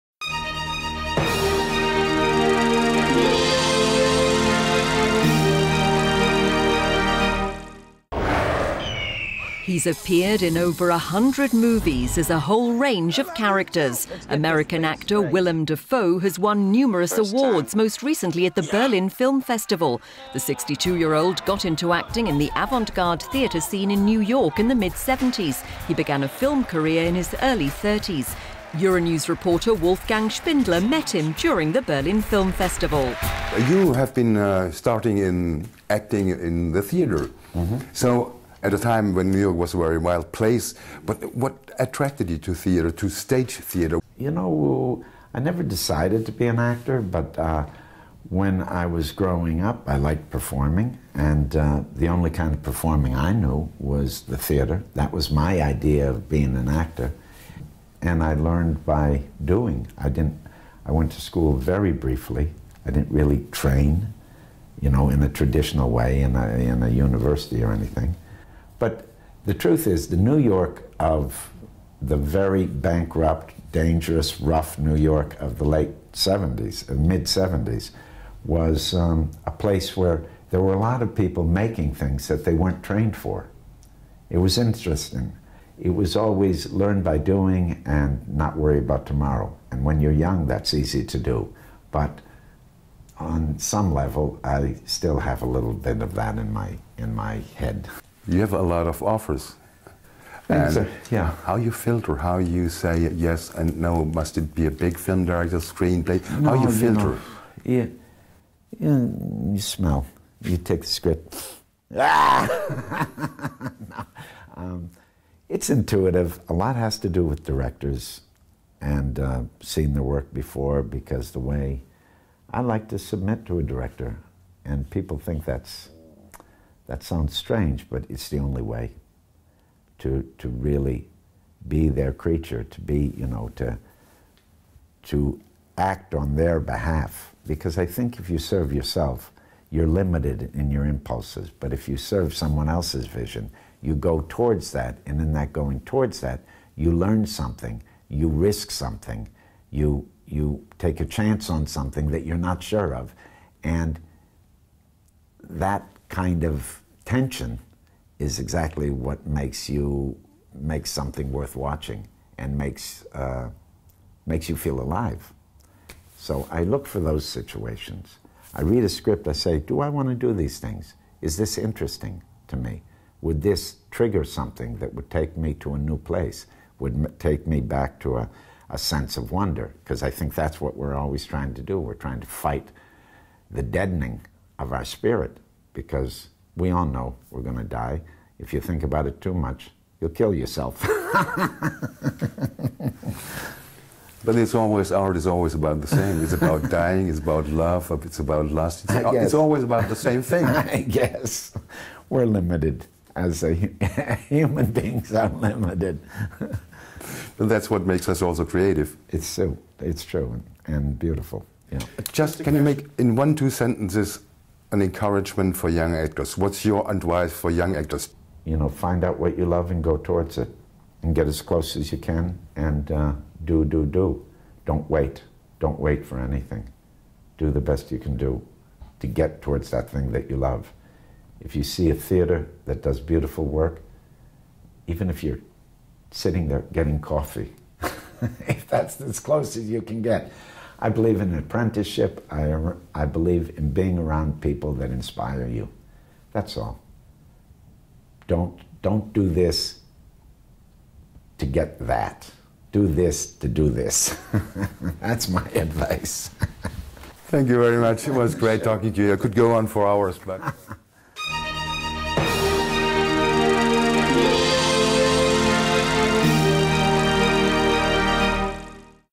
Who is....Willem Dafoe?
Willem Dafoe has had a rich career spanning over one hundred films. His big break came in Oliver Stone's Platoon where he was able to showcase his diverse range as an actor. He got into acting in the seventies in New York's avant guard and experimental theatre scene.

Speaking to Euronews Dafoe said the choice to become an actor wasn't one he made growing up. Instead, he realised that he liked performing in theatre shows. He says he received very little formal education but that during the seve…